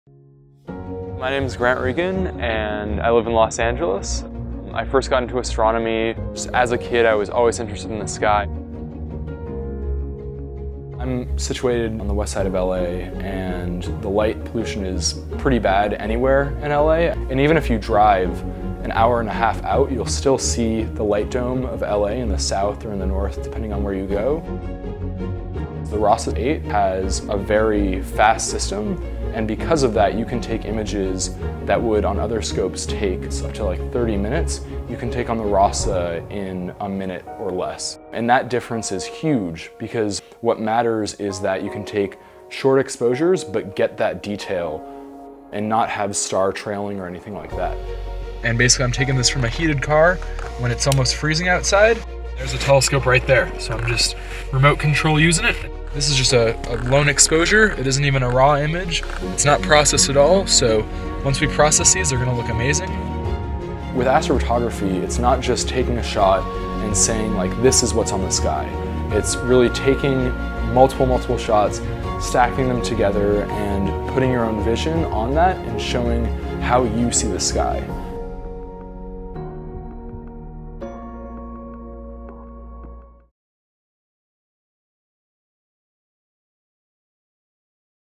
Urban Astroimaging with the RASA 8
Team Celestron member, Grant Regen, takes out the latest Celestron RASA (Rowe-Ackermann Schmidt Astrograph) optical tube to the outskirts of Los Angeles to remotely astroimage from the comfort of his own car.  Grant shares the benefits of the RASA being at f/2 for faster imaging.